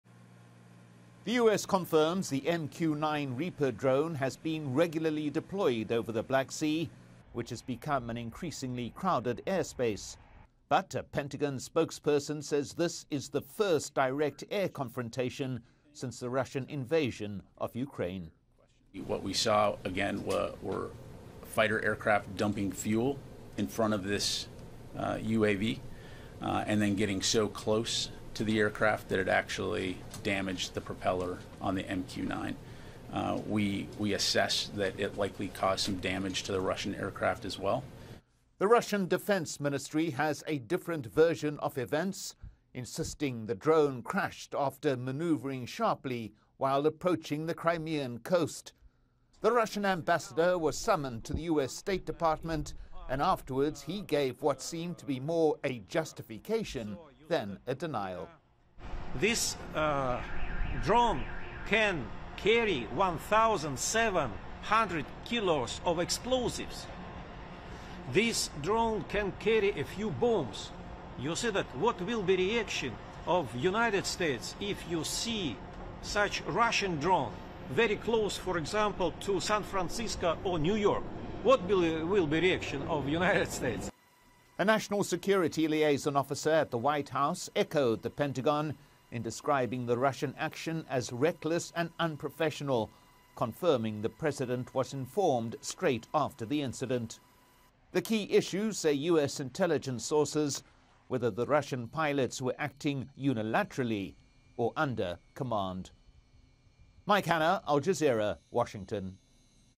Washington decries Russian collision with US drone over Black Sea
The United States military has condemned what it calls a "reckless" manoeuvre by a Russian warplane that brought down one of its surveillance drones over the Black Sea.
The Russian foreign ministry denies the claim, insisting there was no Russian interference before the aircraft crashed on approach to the Crimean coast.

Al Jazeera's Mike Hanna reports from Washington, DC.